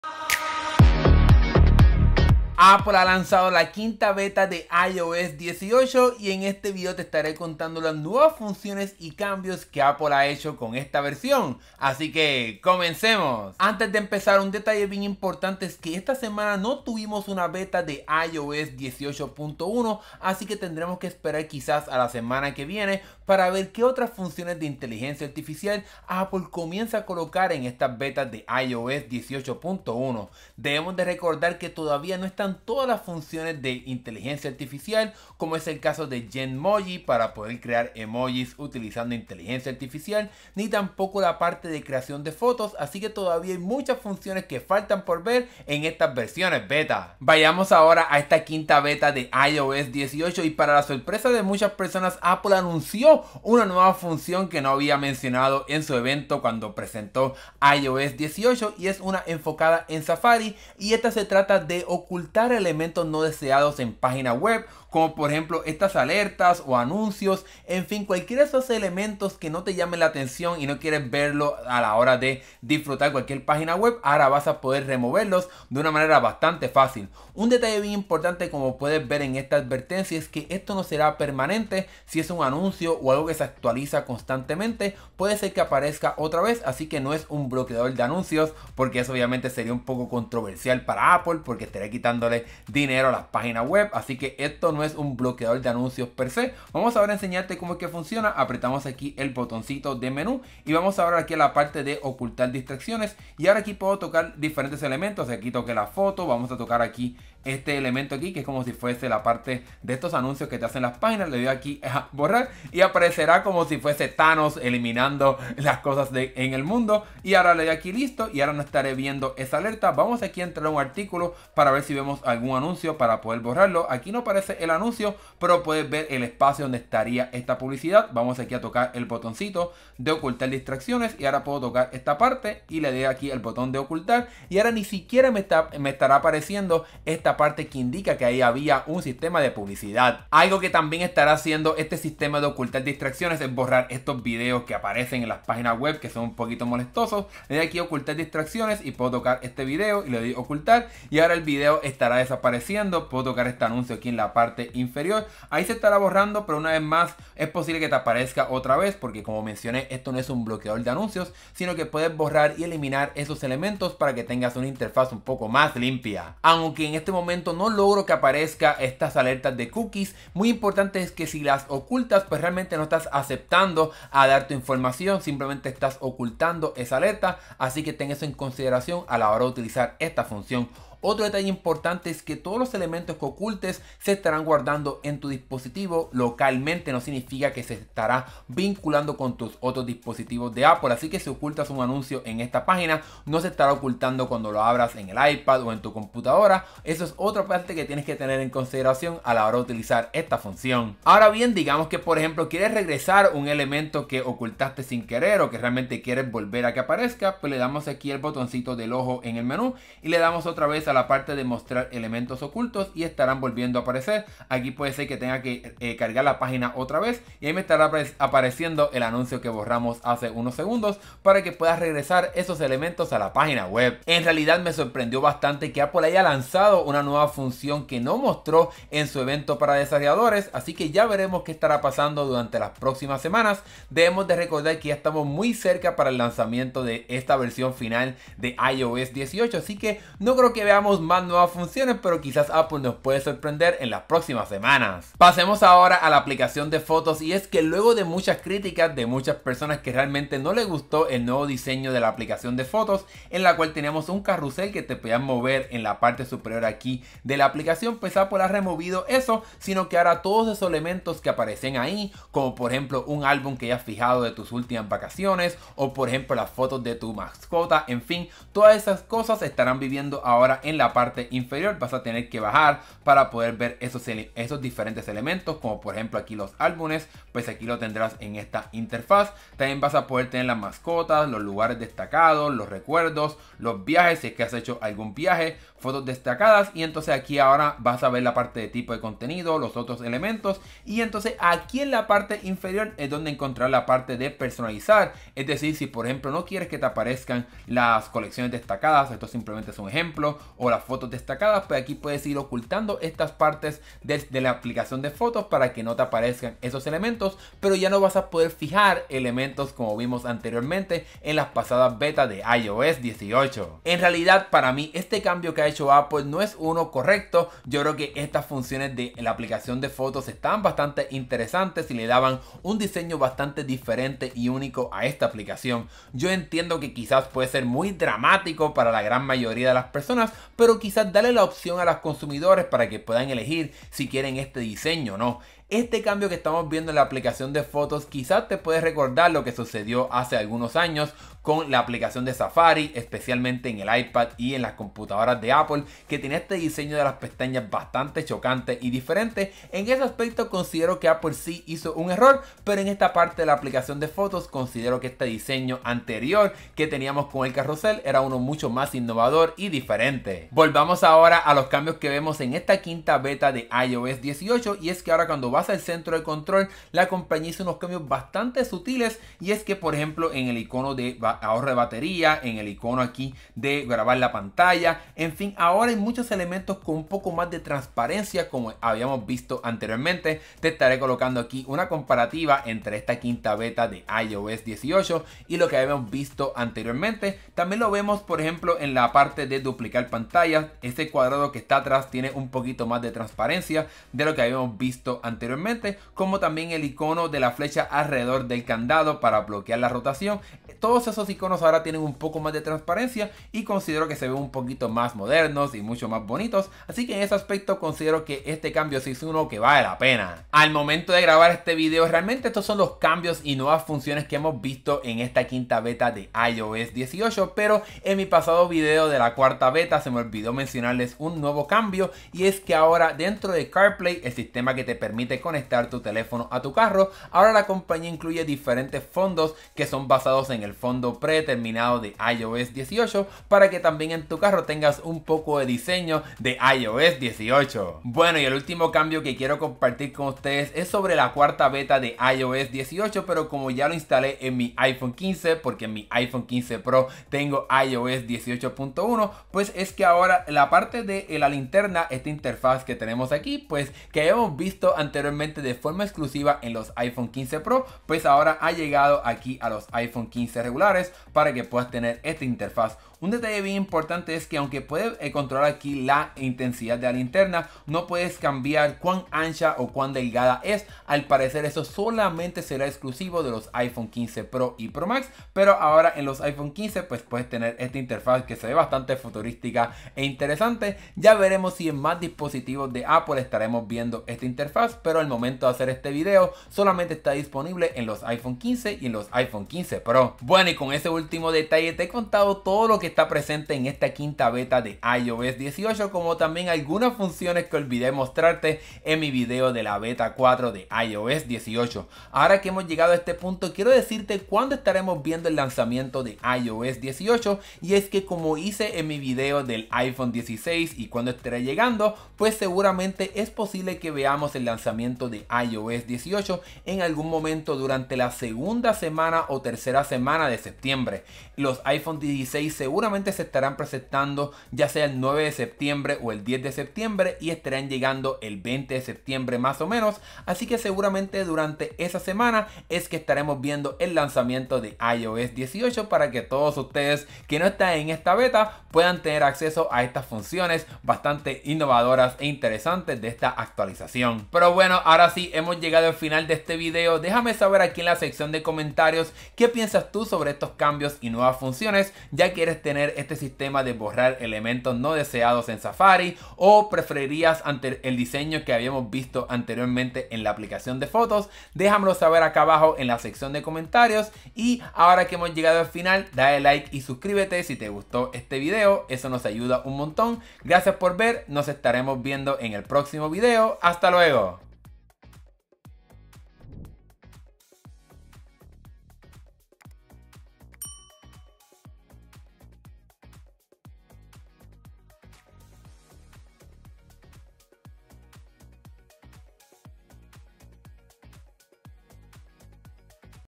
iOS 18 Beta 5 - NUEVO MODO THANOS!! | 9 Cambios Más!
En este video, exploramos los cambios más recientes en la beta 5 de iOS 18 para que conozcas cómo va mejorando y cambiando la próxima actualización que recibirá tu iPhone.

0:00 - Intro
0:13 - Aclaración
0:47 - Ocultar Distracciones en Safari
2:20 - Ocultar Videos Molestosos
2:46 - Ocultar Alertas de Cookies
3:01 - Detalles de Esta Función
3:21 - Regresar Elementos Ocultos
(3:43 - Opinión)
4:05 - Cambios al Diseño de Aplicación de Fotos
(5:13 - Opinión)
6:00 - Diseño del Centro de Control
6:50 - Nuevos Fondos para CarPlay
7:18 - Interfaz de Linterna en iPhone 15 / iPhone 15 Plus
8:10 - Lanzamiento